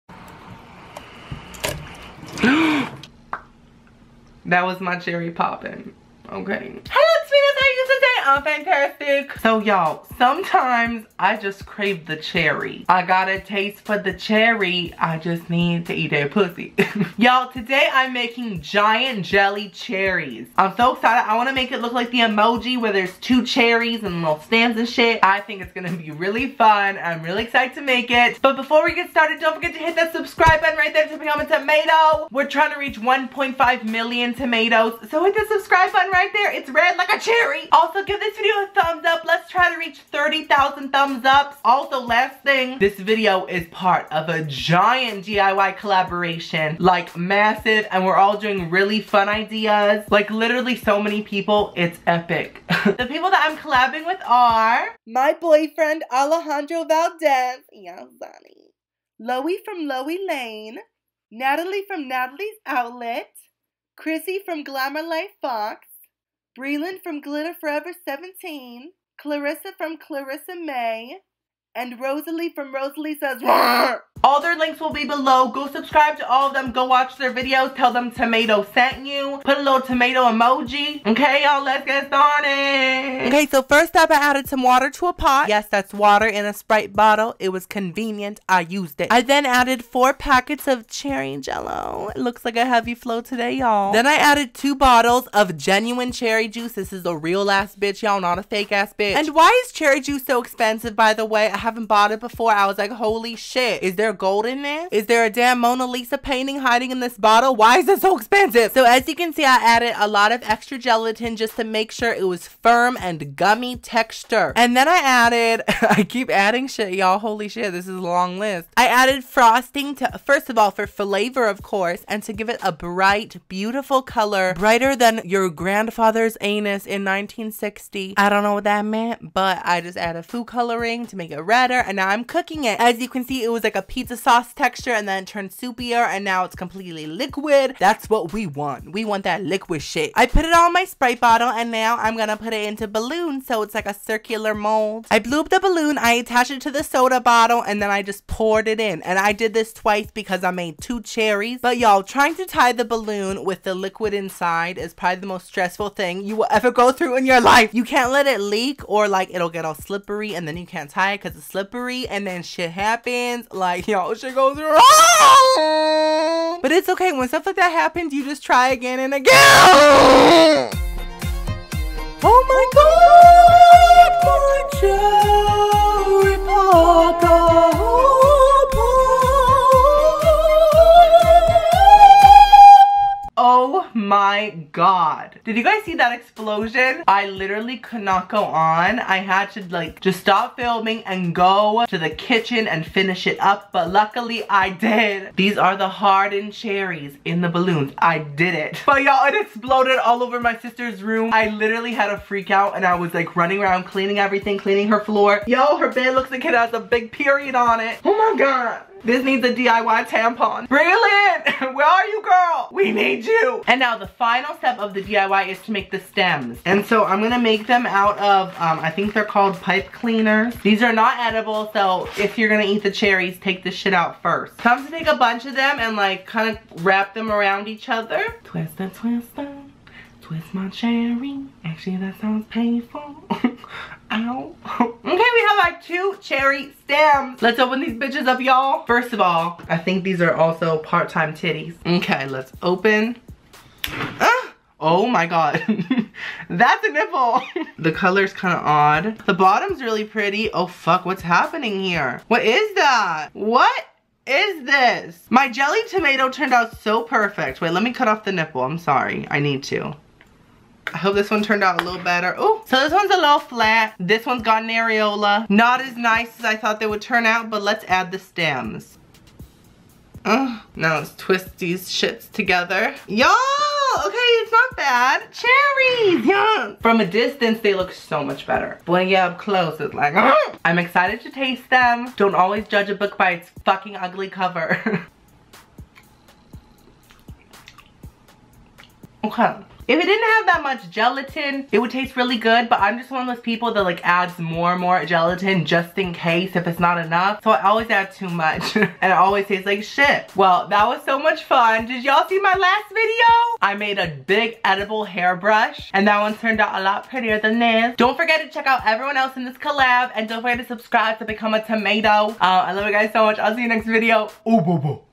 DIY GIANT GUMMY CHERRIES | Huge Edible Jelly Cherry Emoji | Jello Fruit Emojis
Hello Timatoes! so today i made DIY Giant, gummy jelly cherries! it's basically a huge version cherry emoji! It was a disaster to make LMAO but it was a lot of fun! let me know if you guys like DIYs like this!